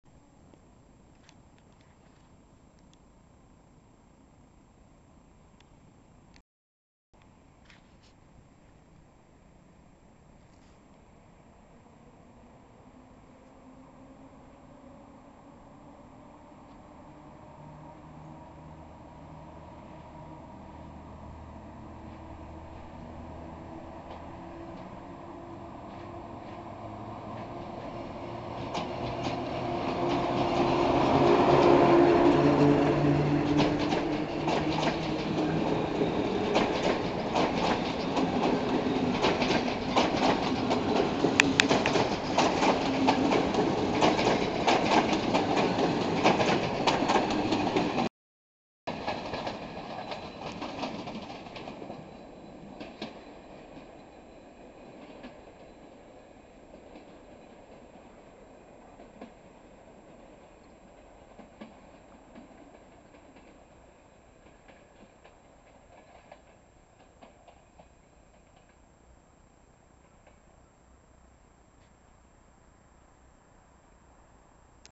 EP07-395 + TLK 83102 opuszczają Kołobrzeg + MK w wagonie :D
Poranny wyjazd dawnej Krakowianki ciągniętej przez zmodernizowaną EP07-395 z Kołobrzegu do Krakowa przez Poznań.
Film dedykuję wracającemu MarkProductionPL z ferii, który filmował tutaj: 0:42  :D